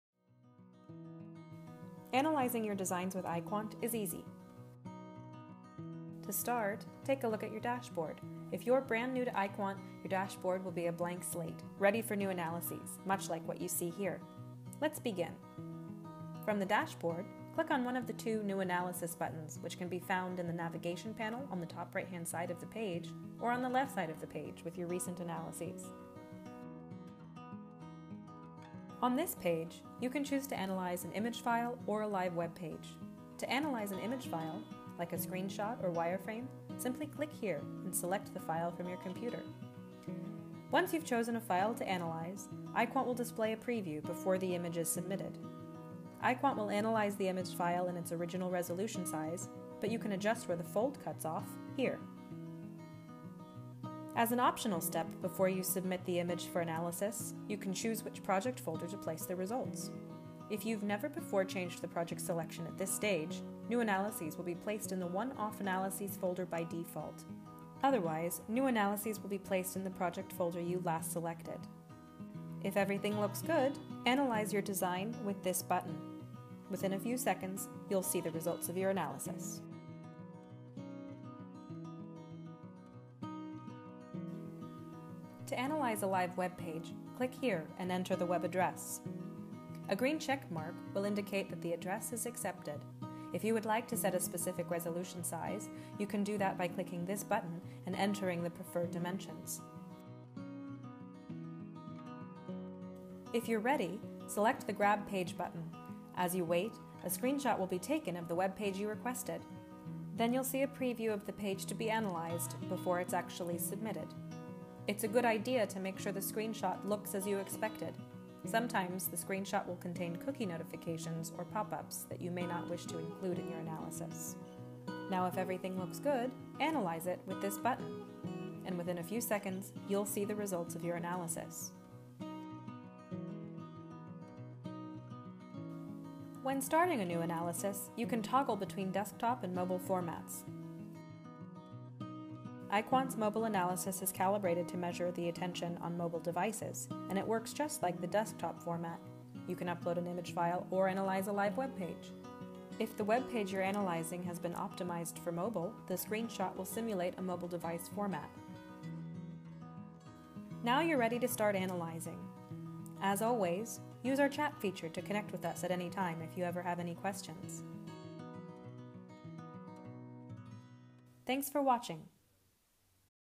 EyeQuant Tutorial: Uploading a Design
Analyzing a design or webpage with EyeQuant is easy!  In this video, learn how to upload an image file for analysis, or use EyeQuant to analyze a live webpage.